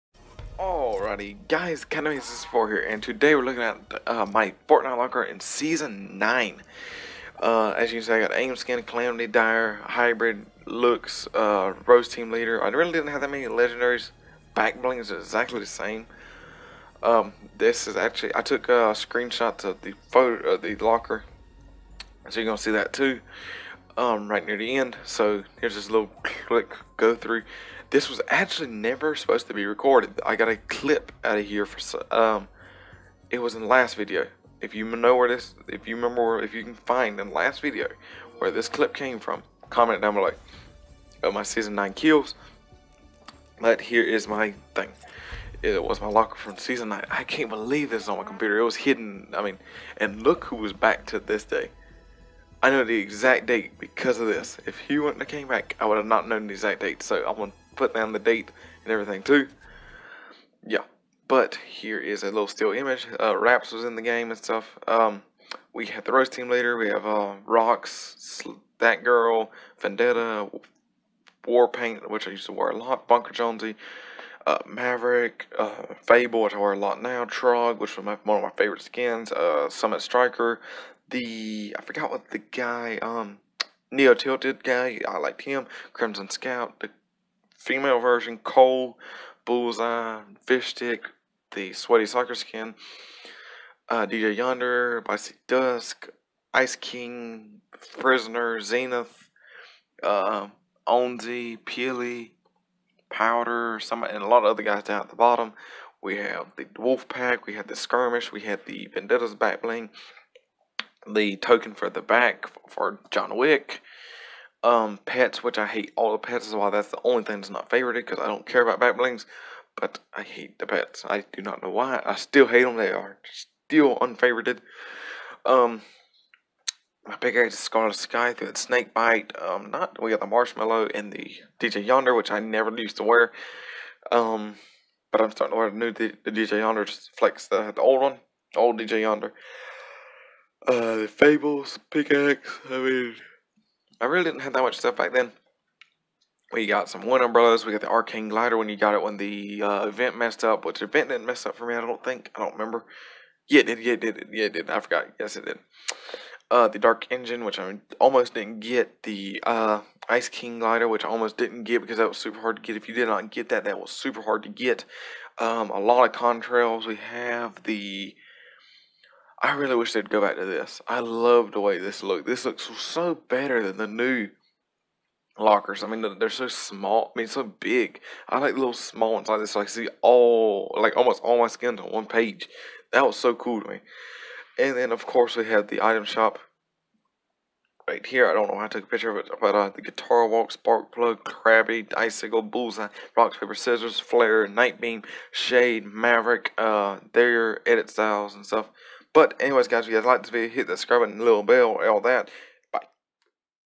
MY LOCKER BACK IN SEASON 9 ( NOT A LOT ) OLD VIDEO
Hey Guy's I Am Captain Mave 64 And In Today's Video We Are looking at my season 9 locker and that is about it so anyway's guy's If You Liked The Video Consider Giving Me A Like And Subscribe
And Comment What You Would Like For Me To Do Next

Gifters Be Here By Gifting Anything